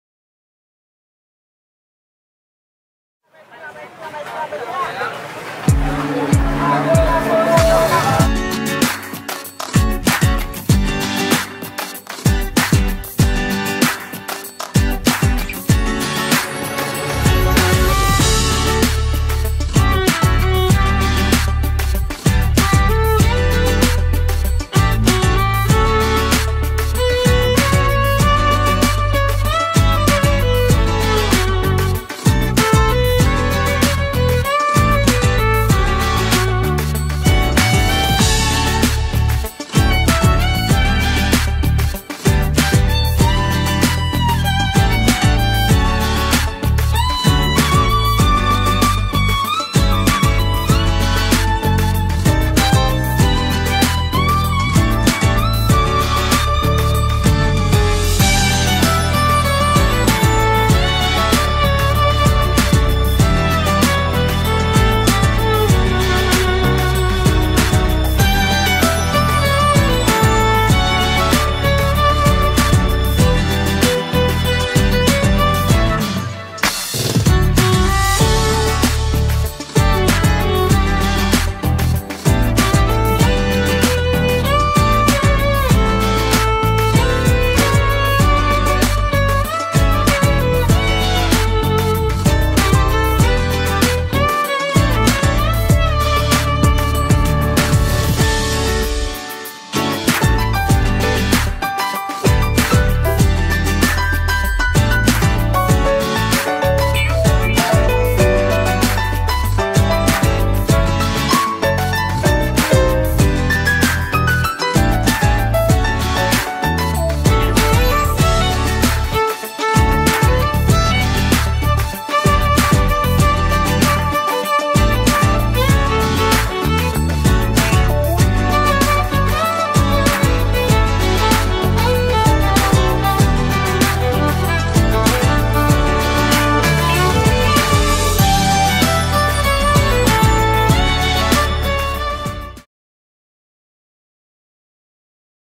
12 November 2022 - Laporan Perkembangan Terkini COVID-19 Di Malaysia
Saksikan Laporan Harian Perkembangan Terkini COVID-19 Di Malaysia yang dibawakan dari Ibu Pejabat Kementerian Kesihatan Malaysia, Putrajaya